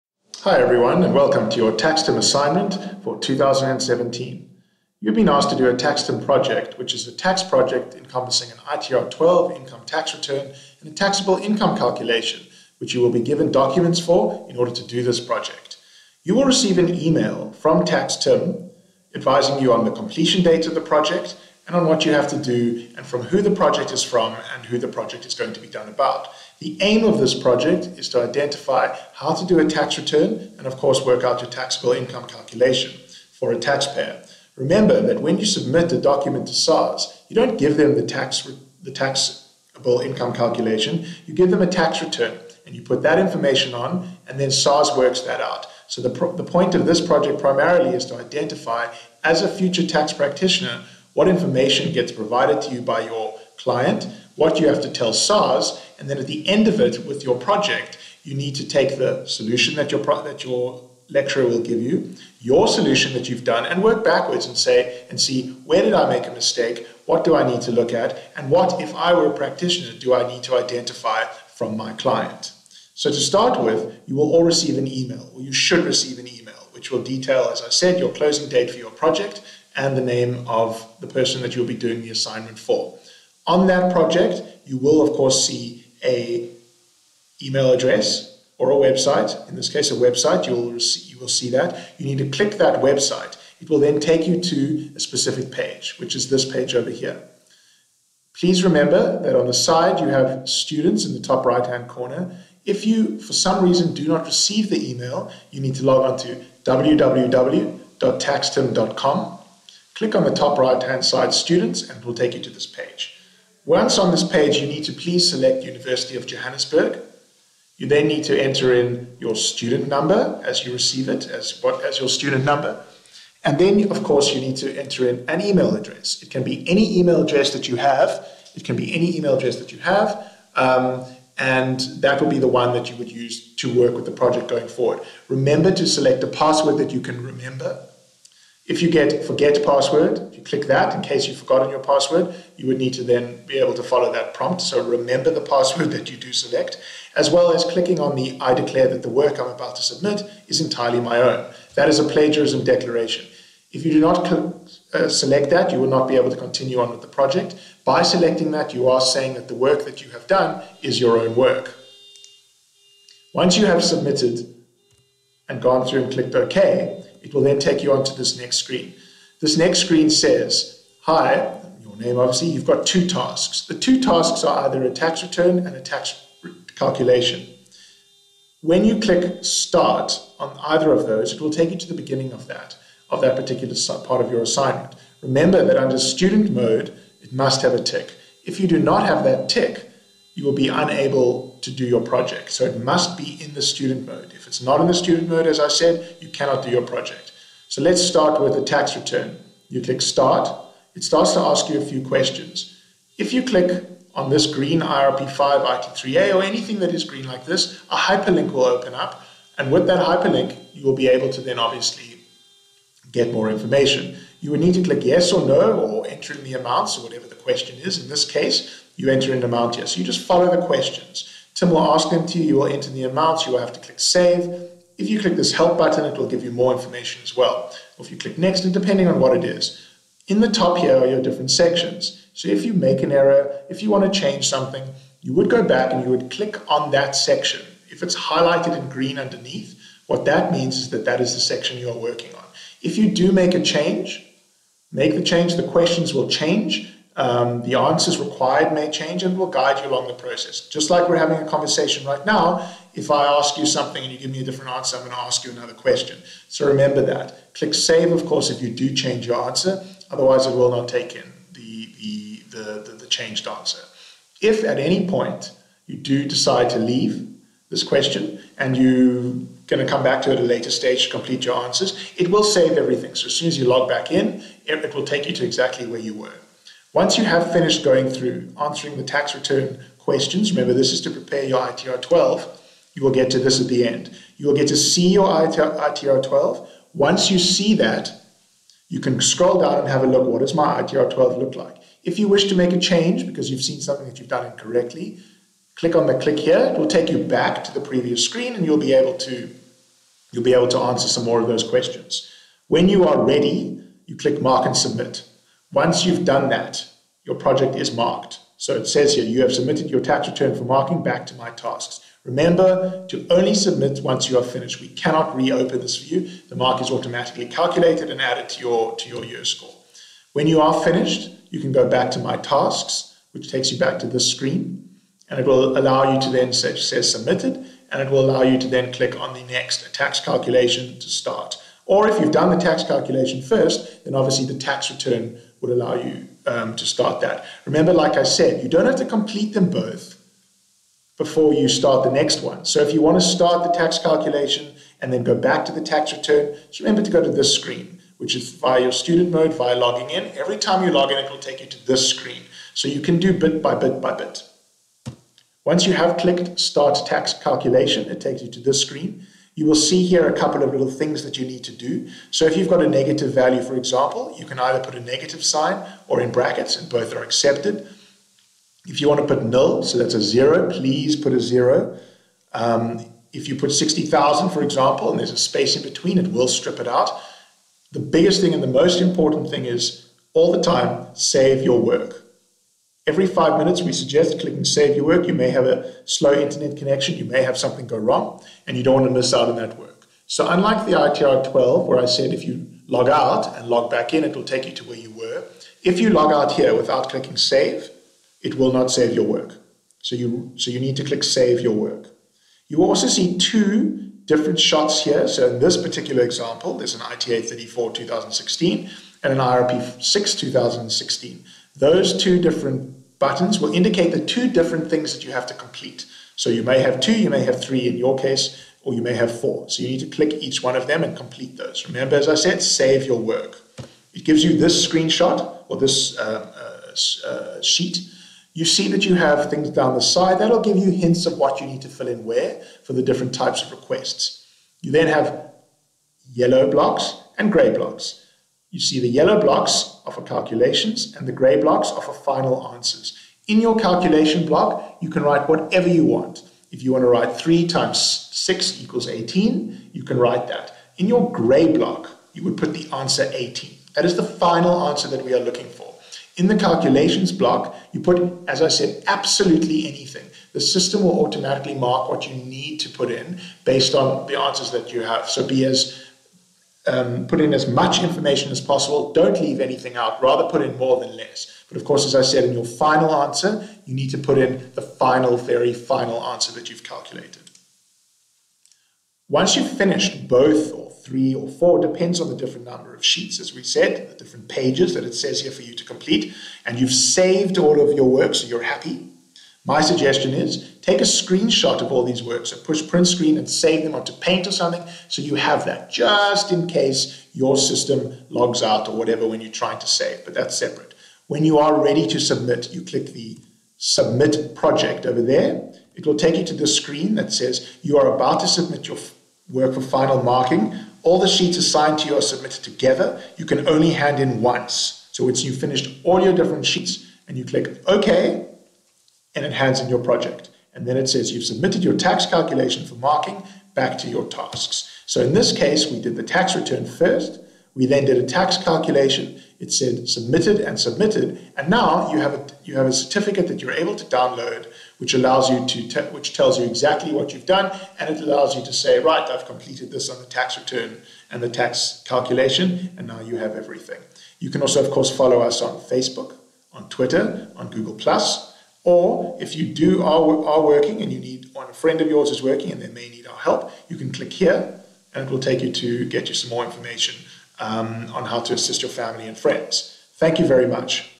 TaxTim Student Assist Walkthrough
This video demonstrates the steps required to complete a project using TaxTim Student Assist.